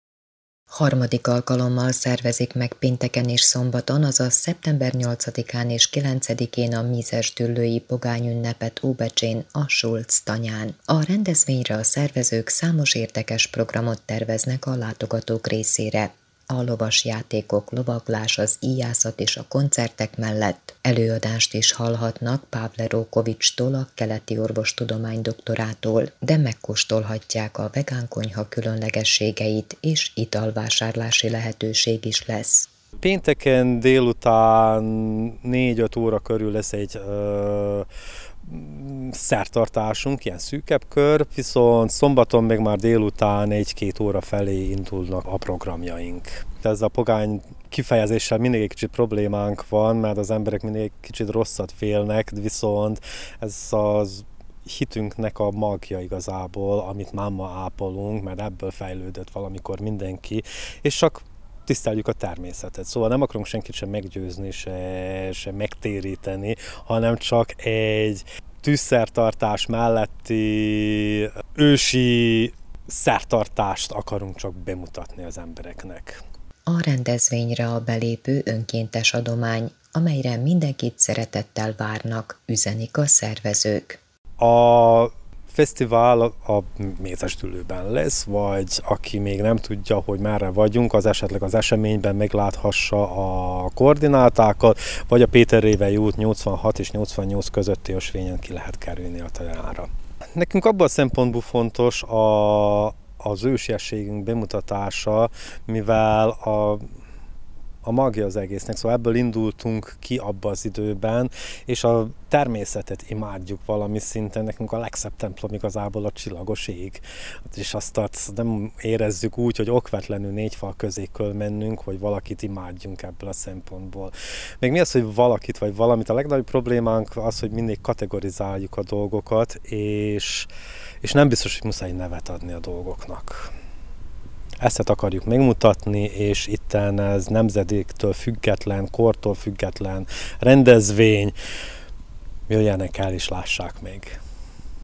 TV BEČEJ: Pénteken és szombaton Mézesdüllői pogány ünnep a Schultz tanyán
TV BEČEJ: Harmadik alkalommal szervezik meg pénteken és szombaton, azaz szeptember 8-án és 9-én, a Mézesdüllői pogány ünnepet Óbecsén, a Schultz-tanyán.